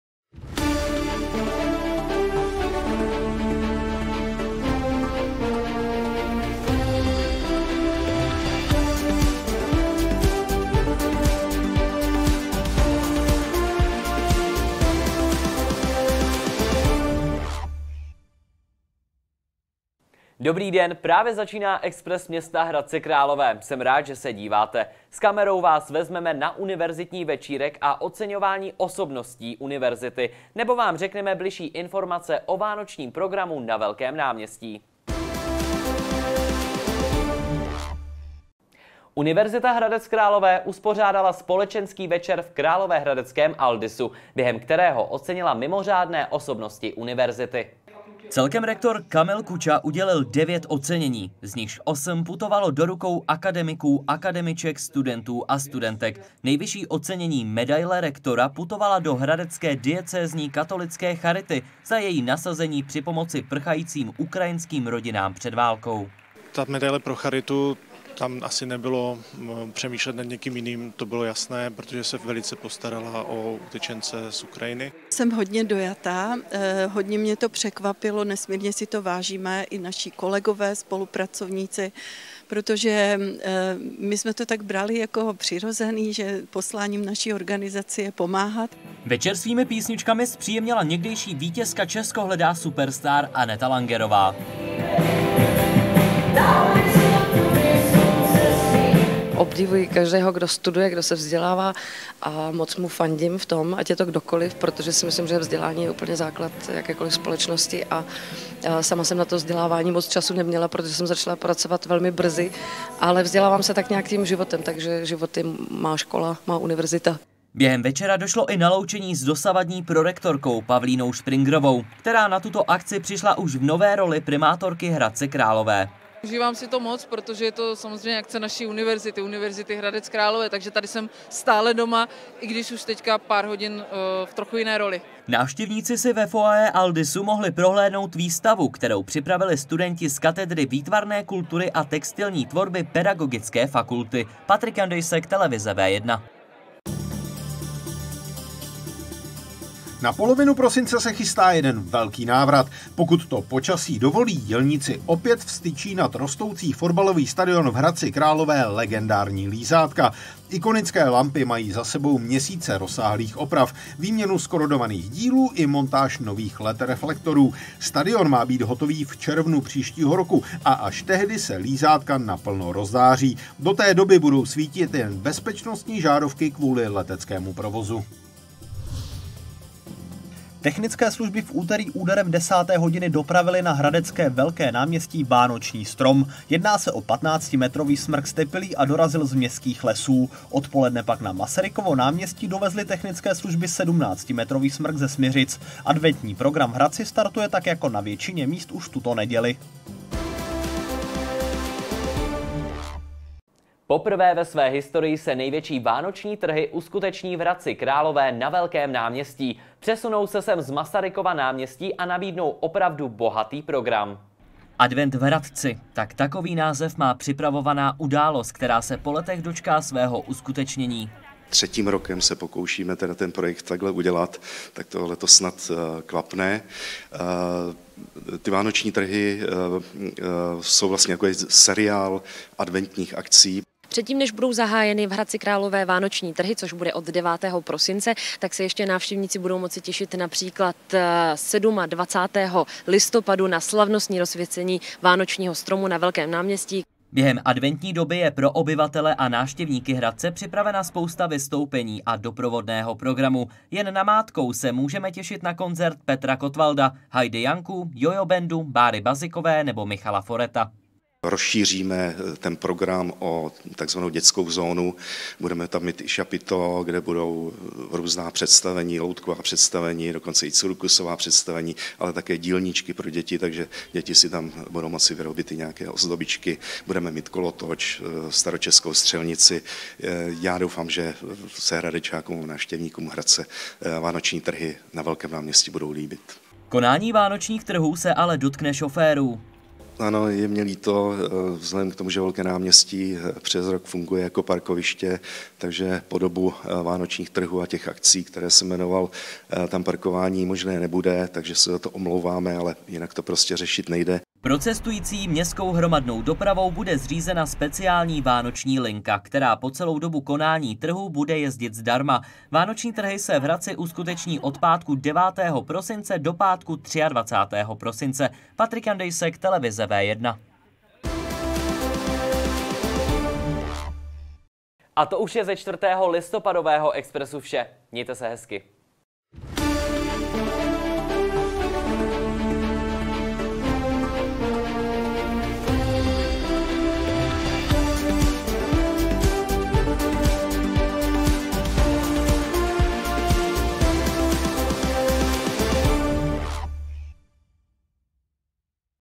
Expres Hradec Králové 46/2022
0:38 Společenský večer UHK
2:29 lízátka na stadionu
2:59 Technické služby dopravily vánoční stromy
3:21 největší vánoční trhy poprvé na Velkém náměstí